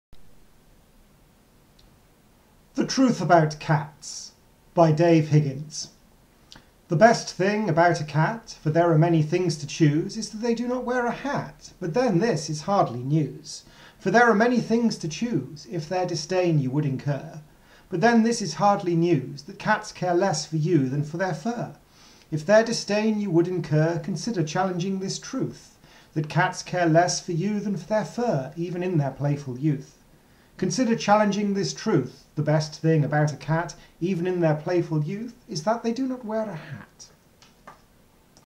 The Truth About Cats by Dave Higgins
A pantoum inspired by my ambivalent feelings towards Dr Seuss.